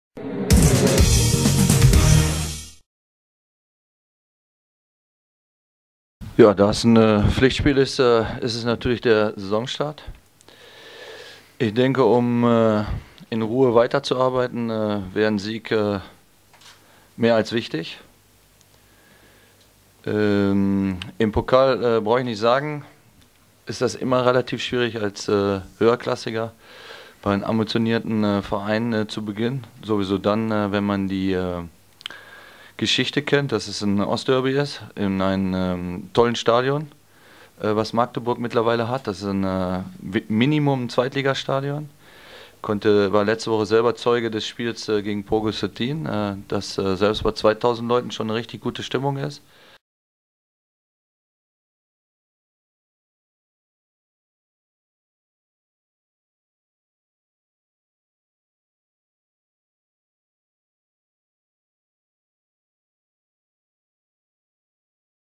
"Ab jetzt: Ehrliche Arbeit!"
"Ehrliche Arbeit auf dem Platz" - das will Pele Wollitz ab Samstag mit dem Saisonstart in jedem Spiel sehen. Den Auftakt macht das Pokalspiel in Magdeburg gegen einen Gegner, den keiner unterschätzt. Die Pressekonferenz auf FCEnergie.tv